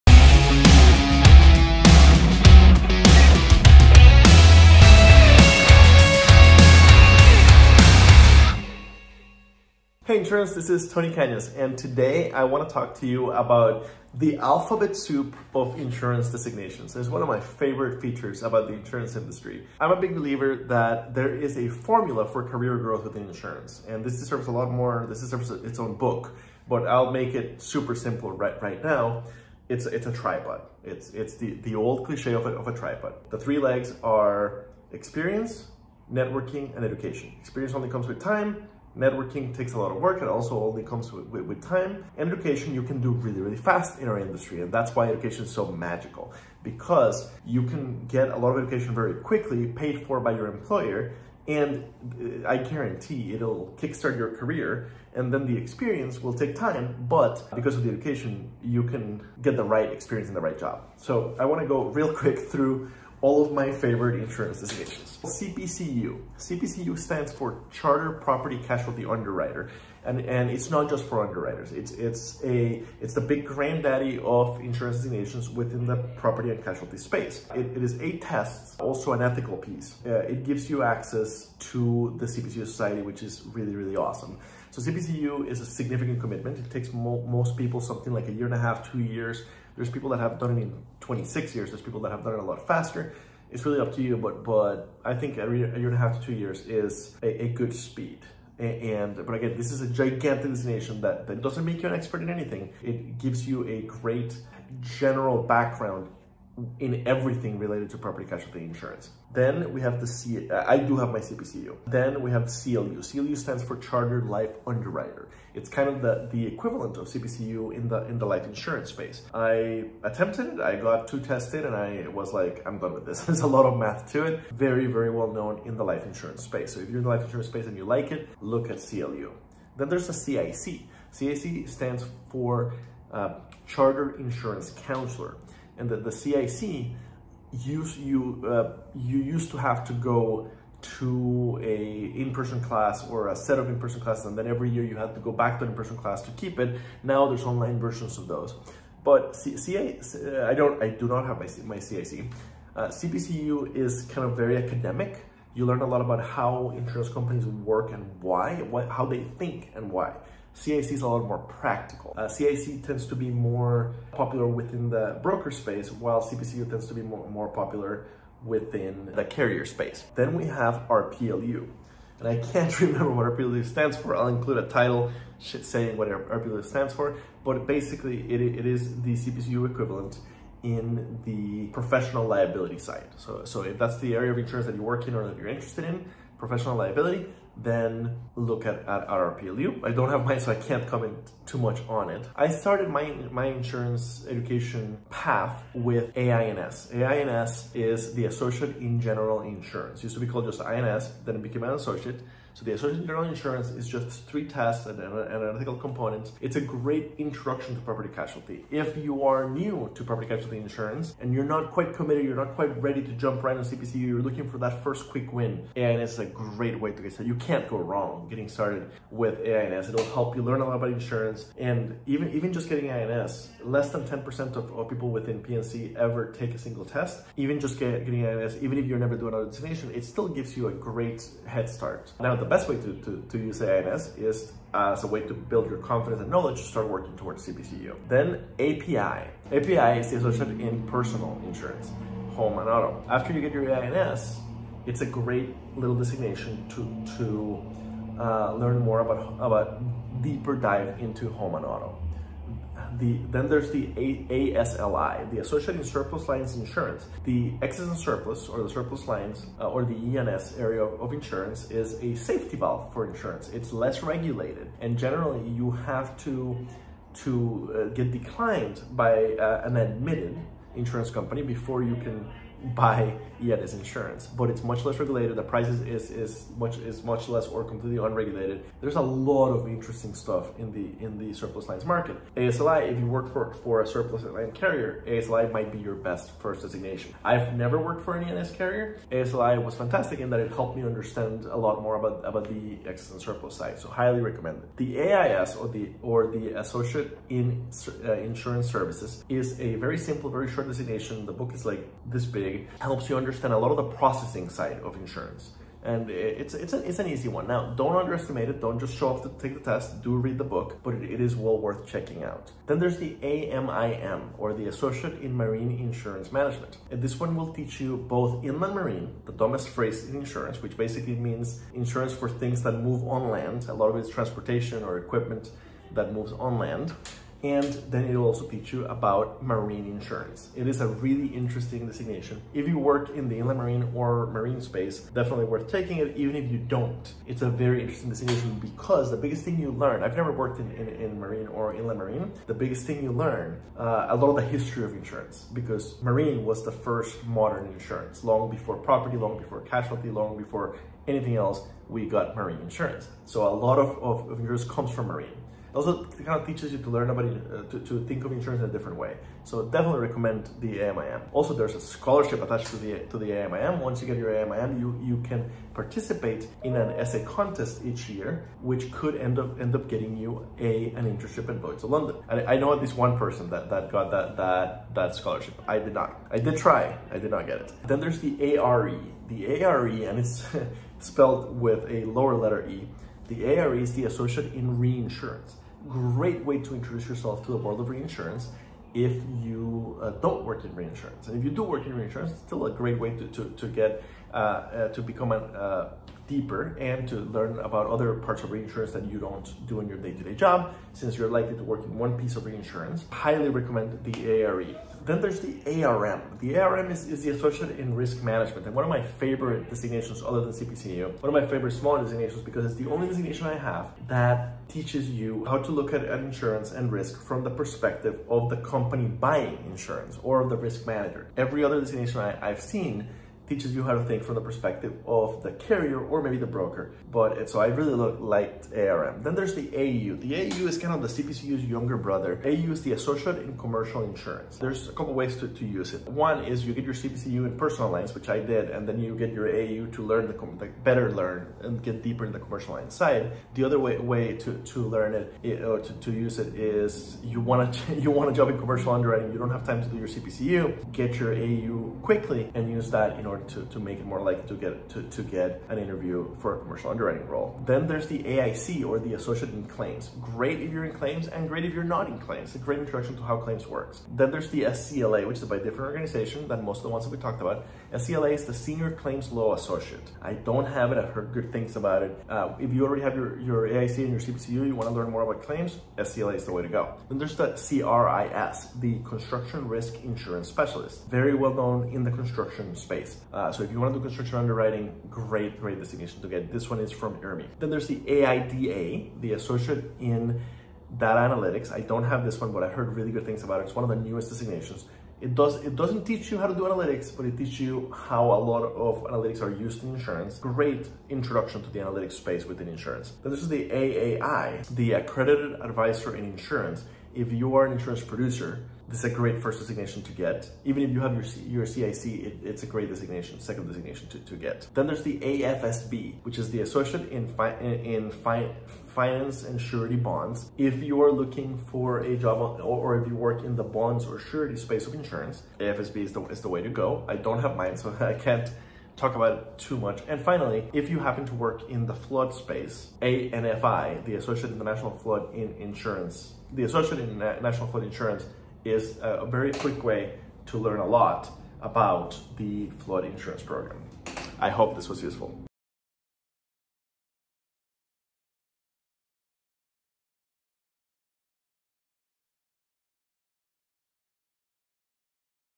The Alphabet Soup of Insurance Designations - Hey Insurance - Ep 112
Hi I'm Tony Cañas, CPCU, AU, ARM, ARe, AIC, AIS, API, ASLI, AMIM, AIC so what exactly do those letters mean? Watch the video to find out and change your own career!